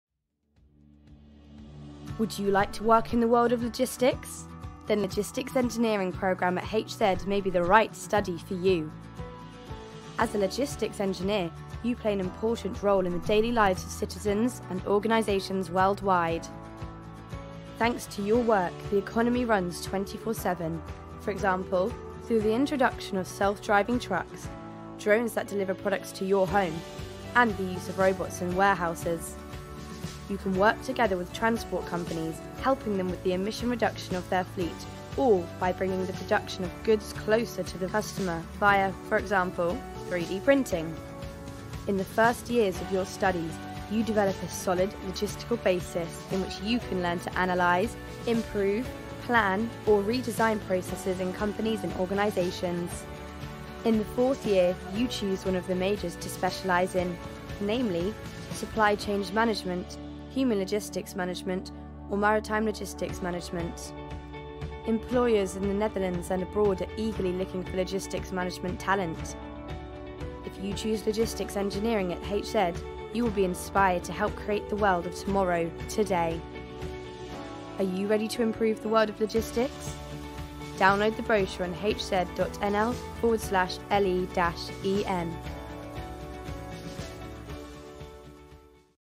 Logistics Engineering explained | HZ University of Applied Sciences
Would you like to work in the world of logistics? Then the Logistics Engineering programme at HZ may be the right study for you.

As a Logistics Engineer, you play an important role in the daily lives of citizens and organizations worldwide.

Thanks to your work, the economy runs 24/7, for example, through the introduction of self-driving trucks, drones that deliver products to your home, and the use of robots in warehouses.

You can work together with transport companies, helping them with the emission reduction of their fleet or by bringing the production of goods closer to the customer via, for example, 3D printing.

In the first years of your studies you develop a solid logistical basis in which you learn to analyze, improve, plan or redesign processes in companies and organizations.

In the fourth year you choose one of the majors to specialise in, namely: Supply Chain Management, Human Logistics Management or Maritime Logistics Management

Employers in the Netherlands and abroad are eagerly looking for logistics management talent.

If you choose Logistics Engineering at HZ, you will be inspired to help create the world of tomorrow today.

Are you ready to improve the world of logistics?

Download the brochure on HZ.nl/le-en
Vind ons ook op: